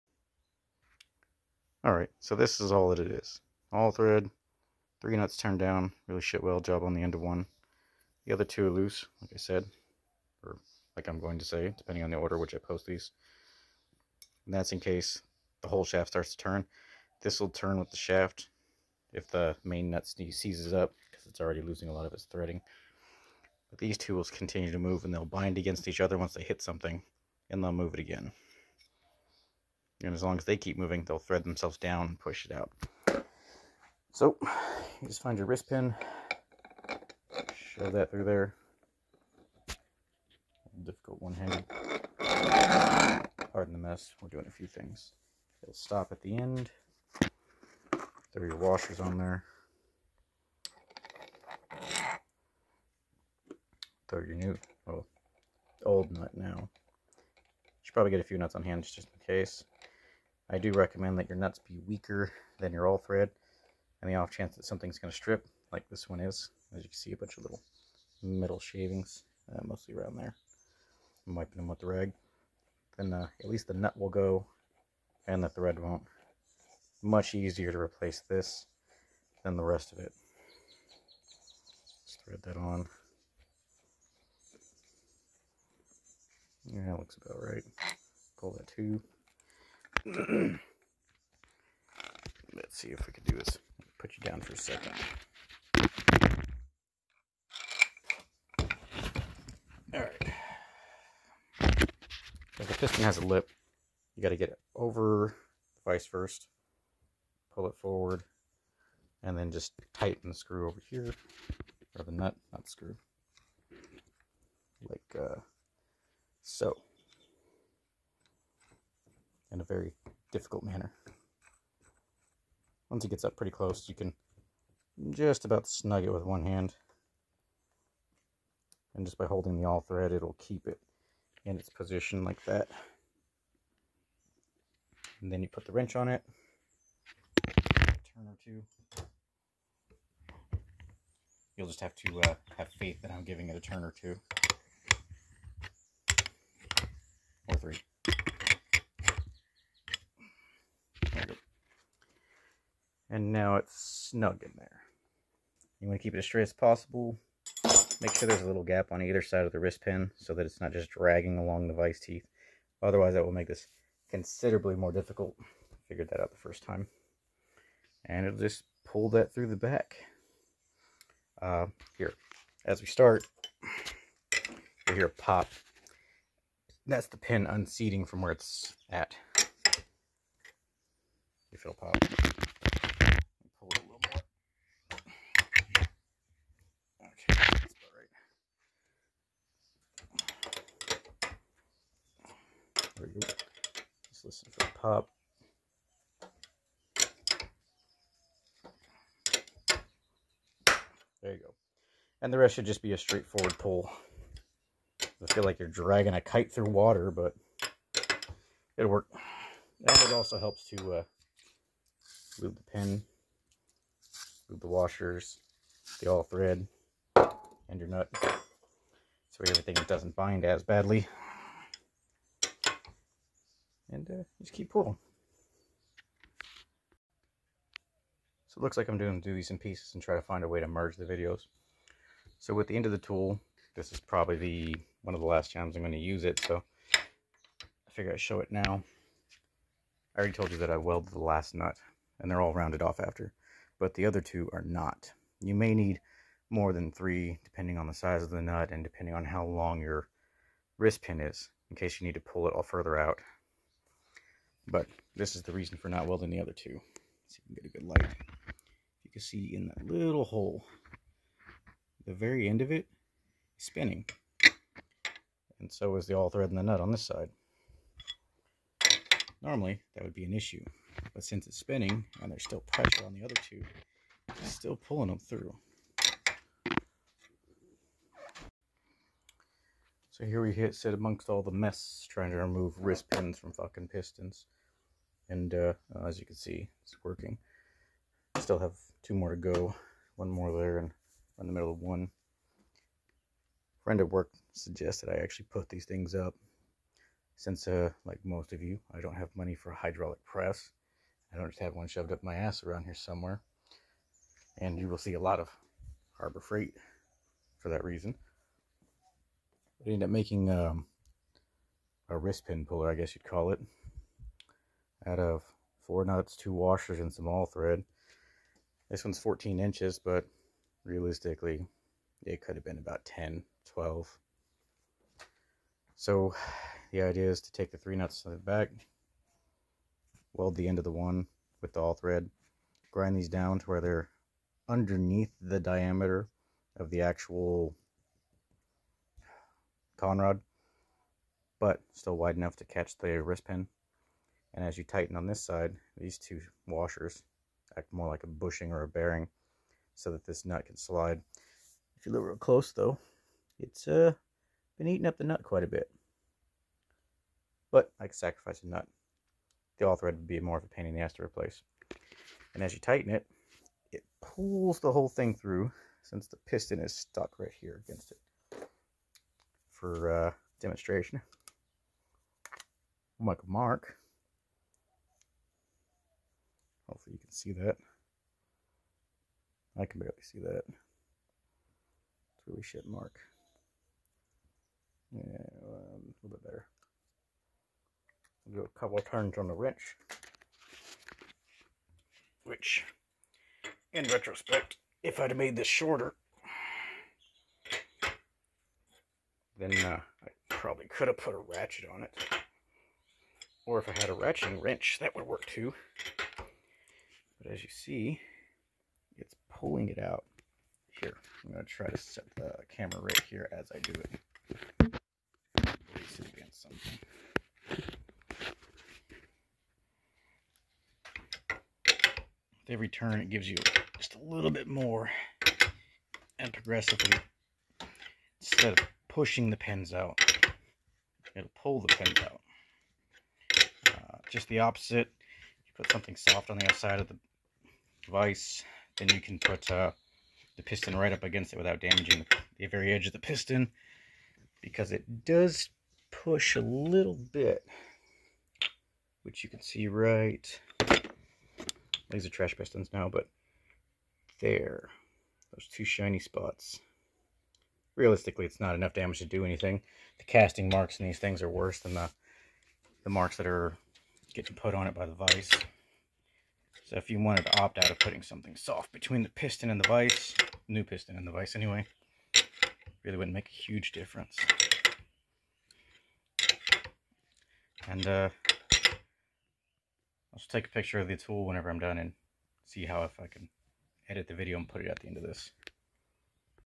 4.9l Connecting Rod Removal and Installation Without a Press
A diy tool I made to remove press fit wrist pins from my connecting rods, hopefully this helps someone else who has more time and patience than money as well.